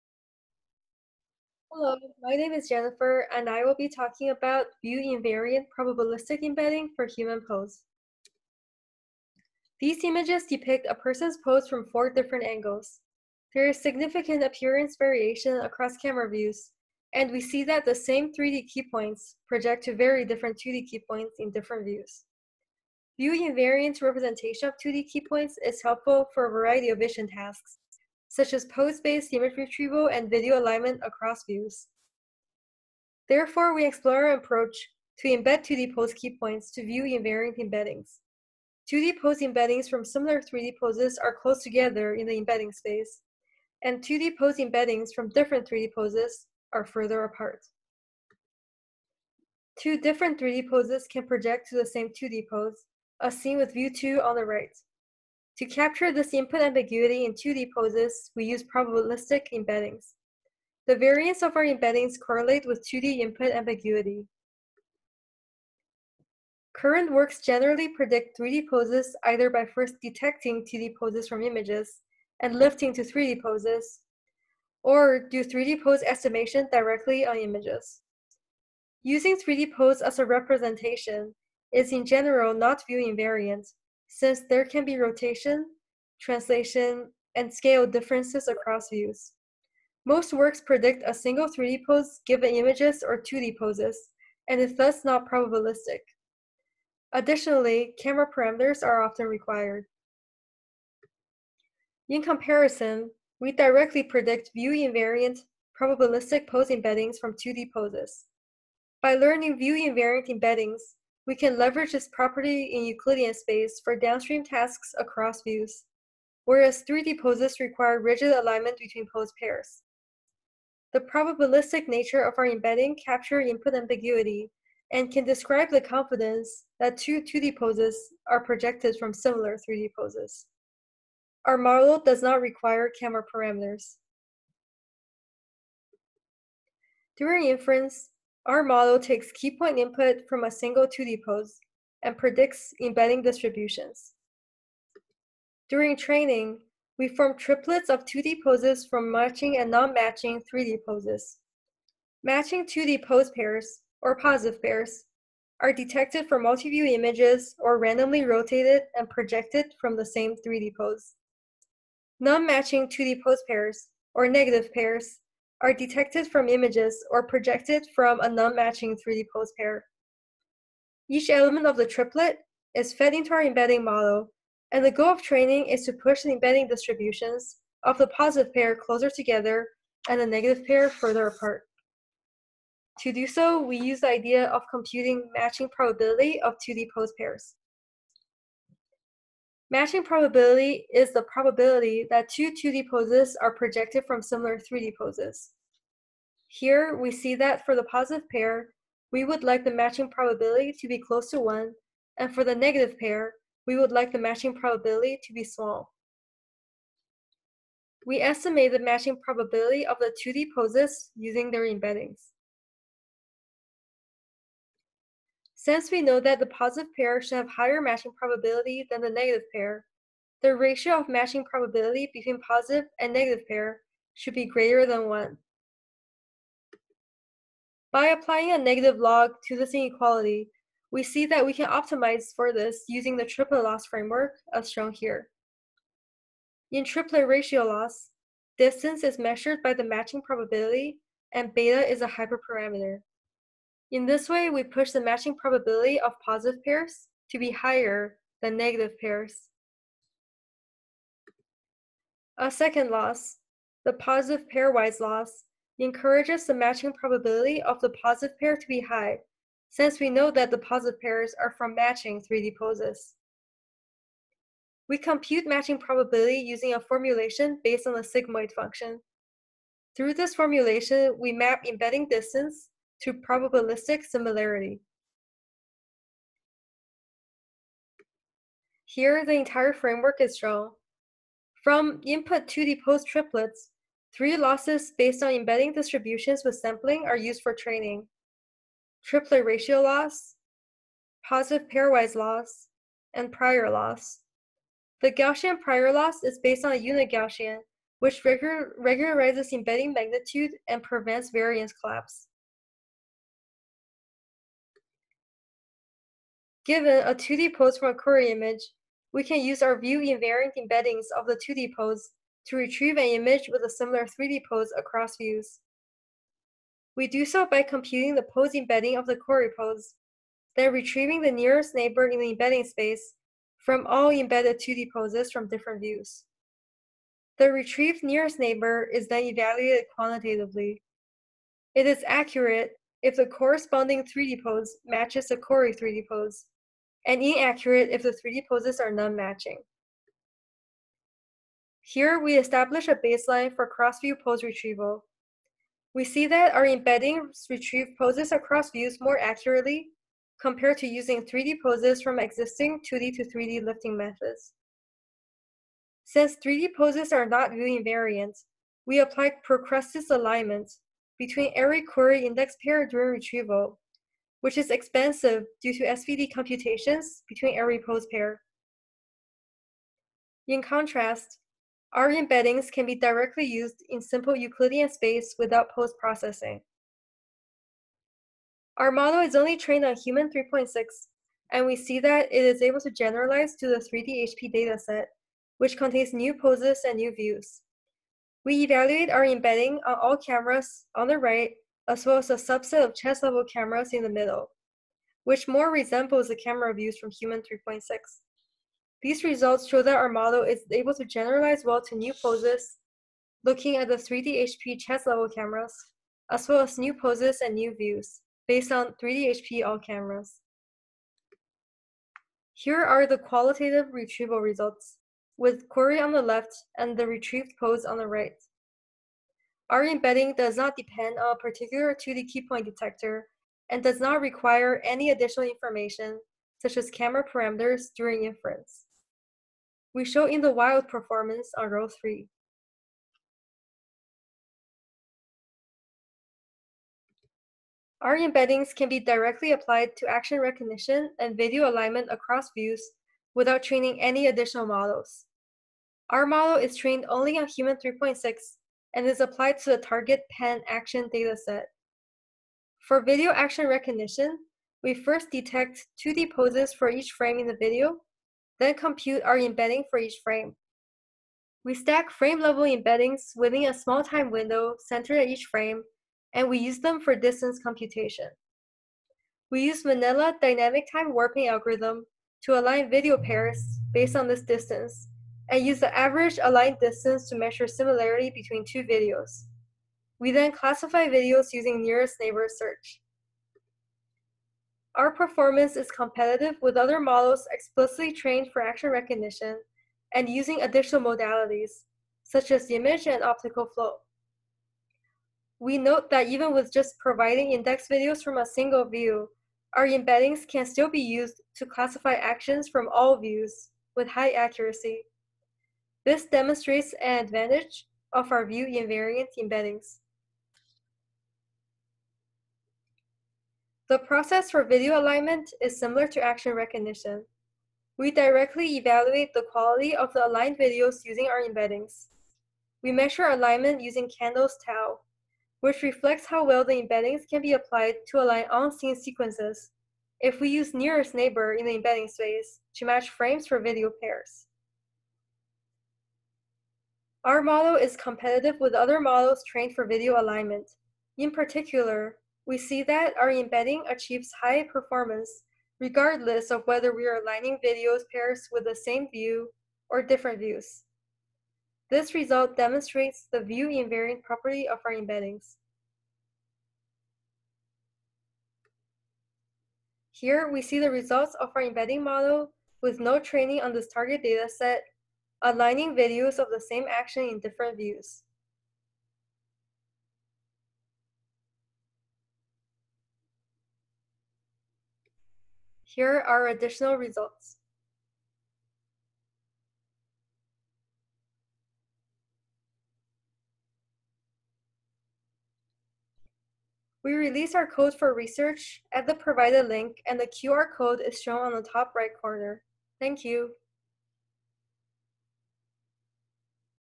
View-Invariant Probabilistic Embedding for Human Pose (ECCV 2020 Spotlight)
Hello! Here is the video presentation for our ECCV 2020 spotlight paper, View-Invariant Probabilistic Embedding for Human Pose. Our model maps 2D poses to a view-invariant probabilistic embedding space, with which we can perform tasks such as cross-view pose retrieval, measuring input ambiguity, action recognition, video synchronization, etc. without additional training. We have released our code and we look forward to hearing about what you can do with pose embeddings!